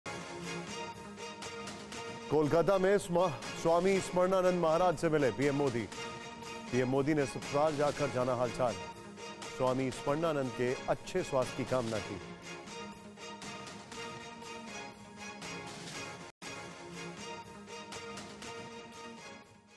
Kolkata में स्वामी स्मरणानंद महाराज से मिले PM Modi, पीएम ने अस्पताल जाकर जाना हालचाल
कोलकाता में स्वामी स्मरणानंद महाराज से मिले PM मोदी, पीएम मोदी ने अस्पताल जाकर जाना हालचाल. स्वामी स्मरणानंद के अच्छे स्वास्थ्य की कामना की रामकृष्ण मठ के अध्यक्ष हैं स्वामी स्मरणानंद.